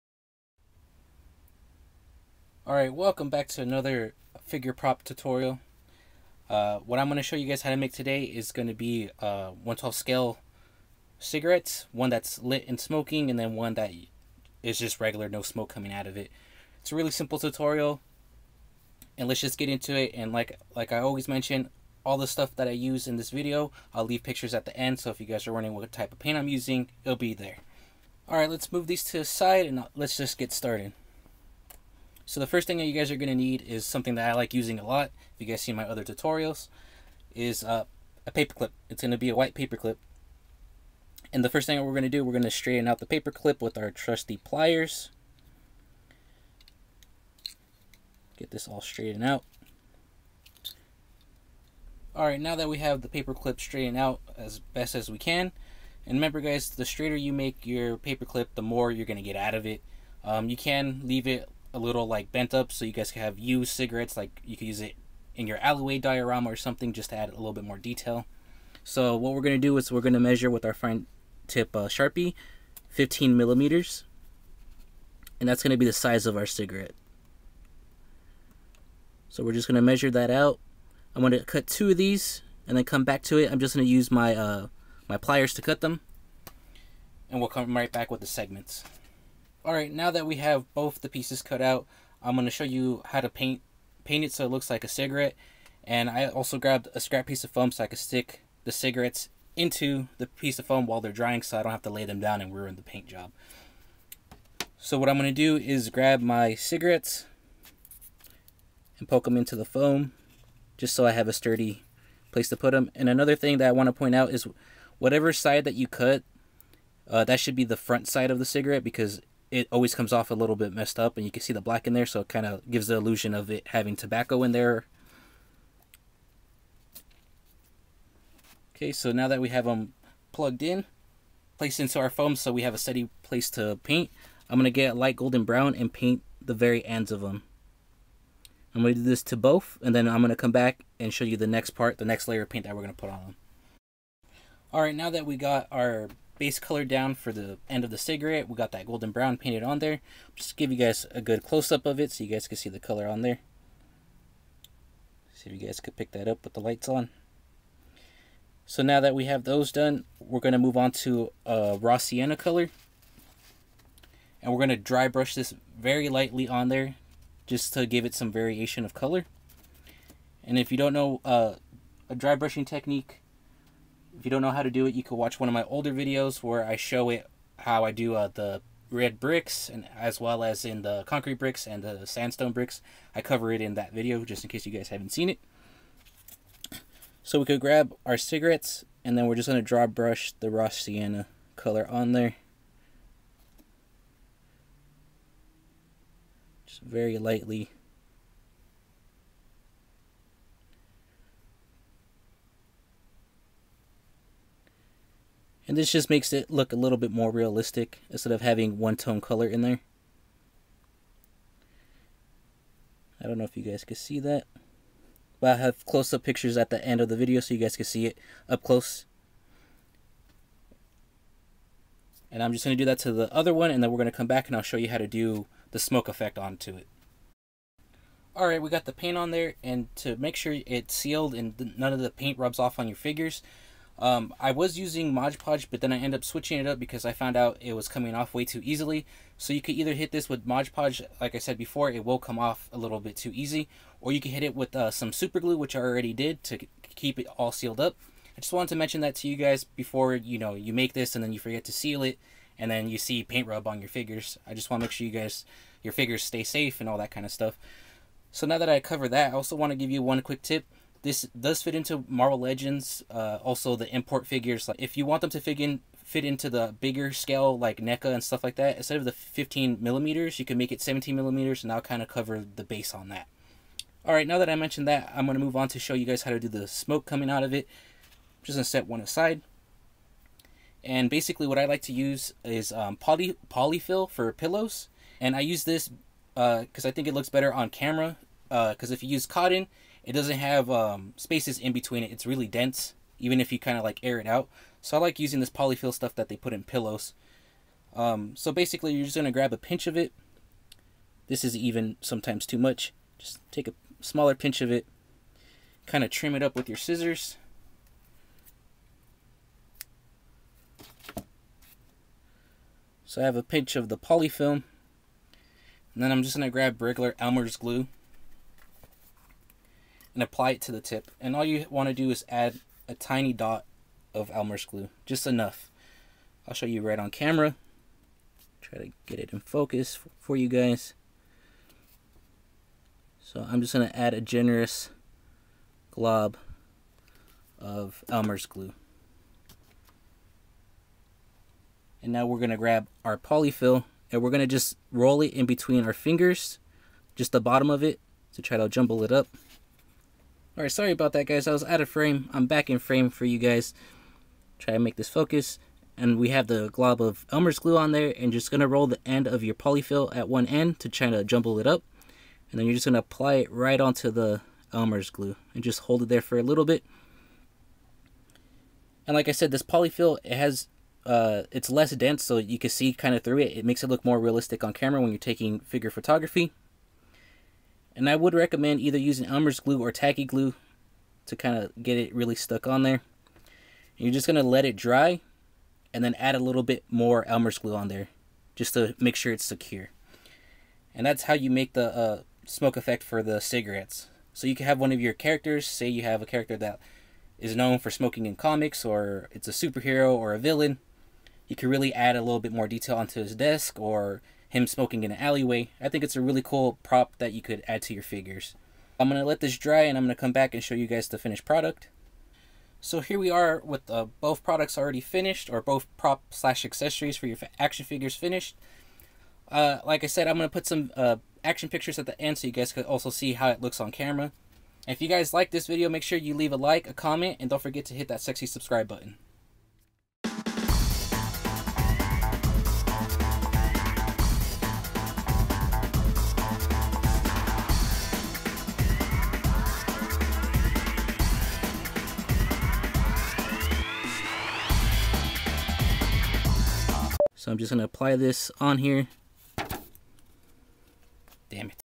1:12 Scale Cigarette Figure Prop
Watch my latest 1:12 scale figure prop/accessory tutorial. I show you how to make 1:12 scale cigarettes with smoke effect for your action figures.

Want me to make something I haven't covered yet leave a suggestion in the comments I read them all!